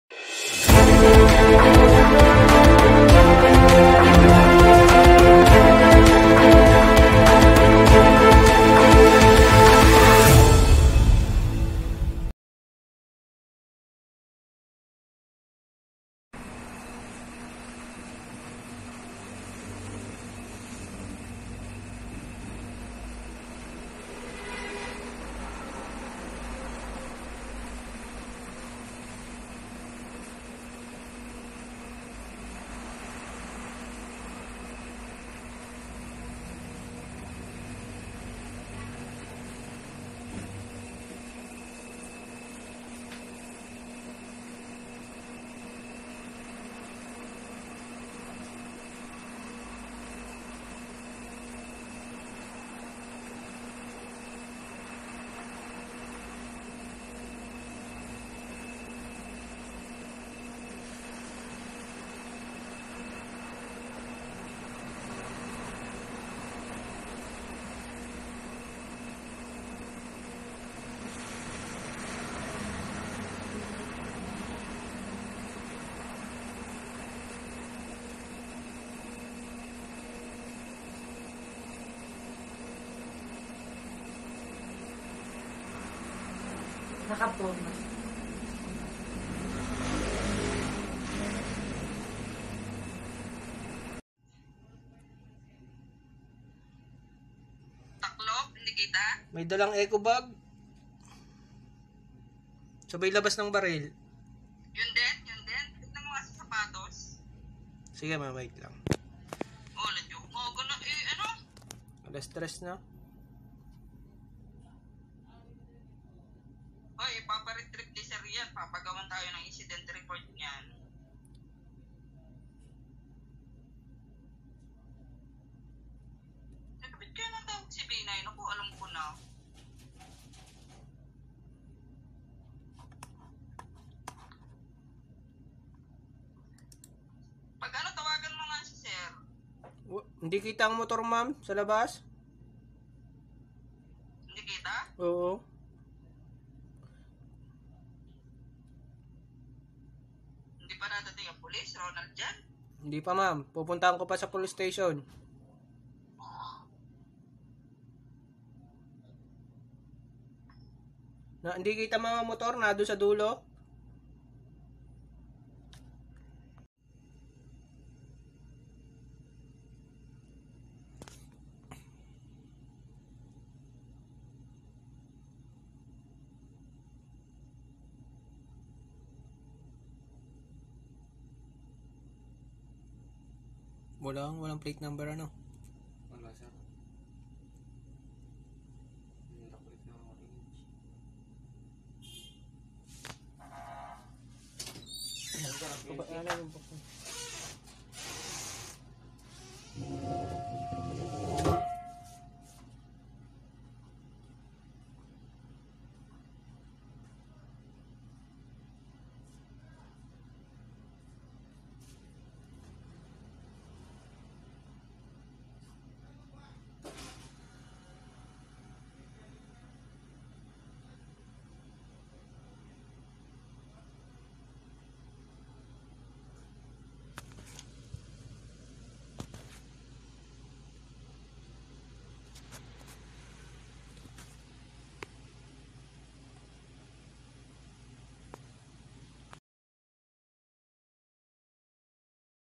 CCTV footage ng naganap na robbery holdup sa Uni Oil sa Sto Tomas, Batangas noong Pebrero 16, 2022
CCTV footage ng insidente ng robbery holdup sa UniOil sa Sto. Tomas, Batangas noong Pebrero 16, 2022 kung saan ang suspek na holdaper na pulis ay naharang at naaresto ng Laguna PNP sa checkpoint sa Pagsanjan, Laguna.